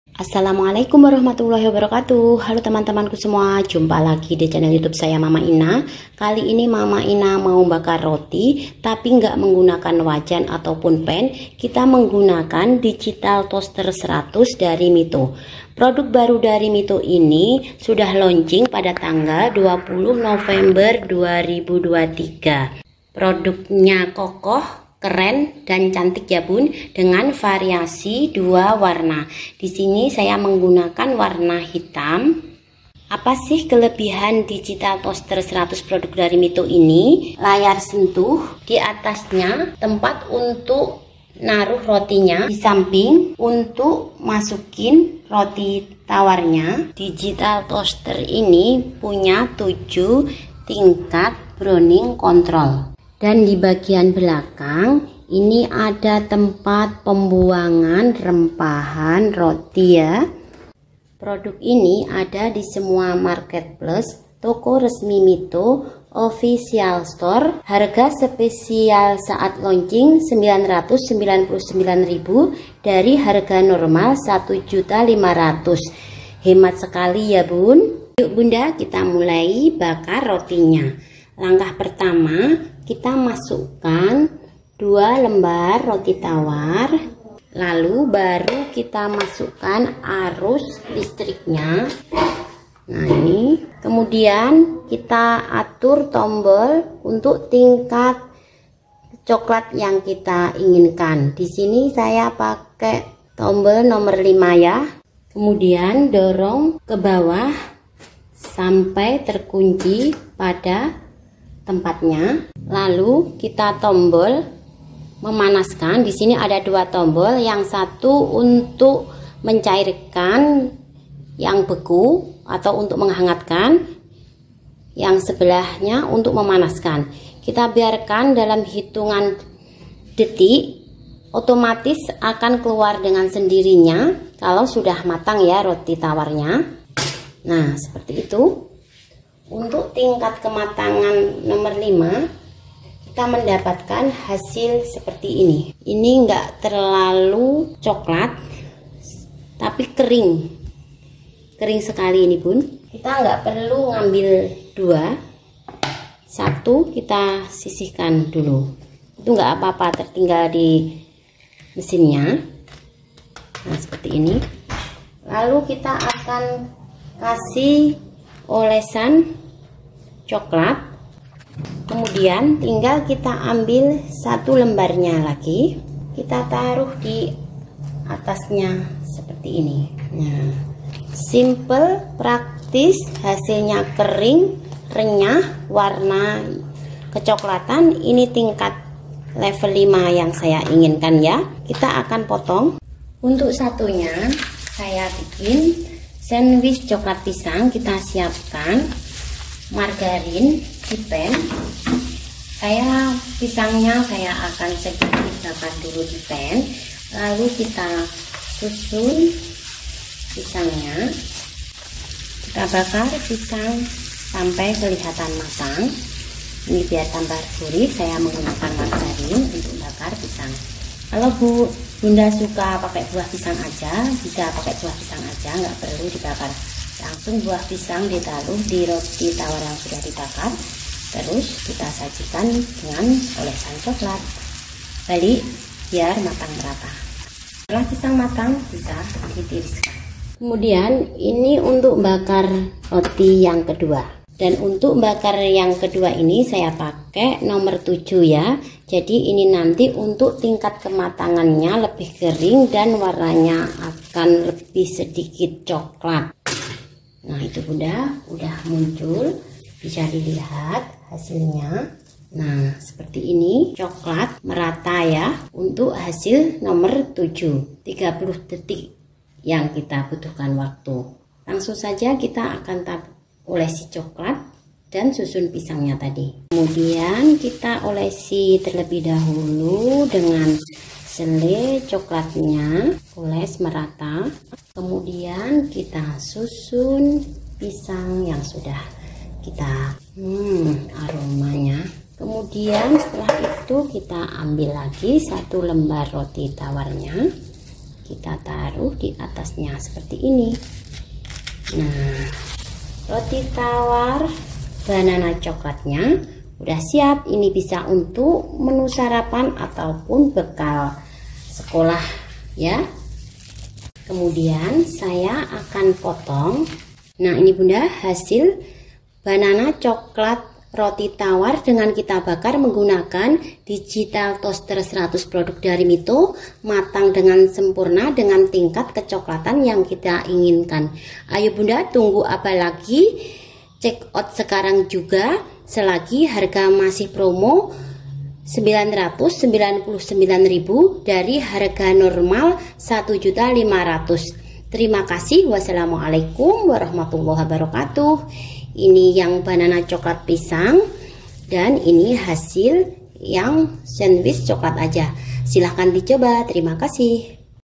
Olahan Roti tawar simpel dengan digital Taoster
Harga : 999.000

digital toast 100

Alat panggang yang simple dan elegant serta mudah digunakan. Memiliki kontrol digital dengan layar sentuh & 7 pengaturan browning control

Spesifikasi
- Kapasitas : 2 slot/slice
- Pemanas : Wire Heating
- Temperatur : -+ 200°C
- Knob unit : 1 Knob (Pengaturan waktu)
- Kontrol Panel : Sentuh & Knob
- Daya Listrik : 800 Watt
- Frekuensi : 220V - 50Hz
- Bahan : PP & Pemanas Element Wire Heating (Alchrome)
- Lampu : Digital LED Display
- Protector : Fuse Resistence Protection for Over current & auto off for time
- Fungsi : Cancel, Defrost, Reheat
- Aksesoris : Crambtray / Alas pembuangan sisa bekas memanggang